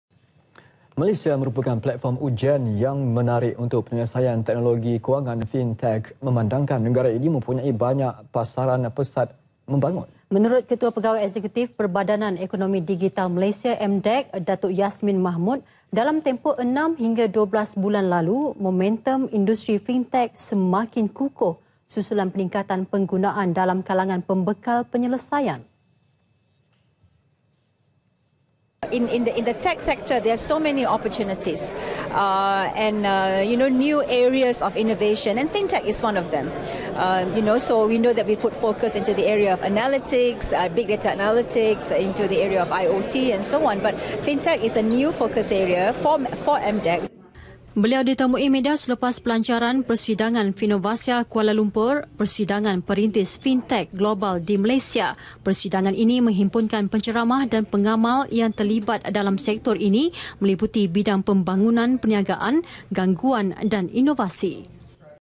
MDEC - MALAYSIA MERUPAKAN PLATFORM UJIAN (FinTech) [20 MAC 2017]
Malaysia merupakan platform ujian yang menarik untuk penyelesaian teknologi kewangan (FinTech) memandangkan negara ini mempunyai banyak pasaran pesat membangun.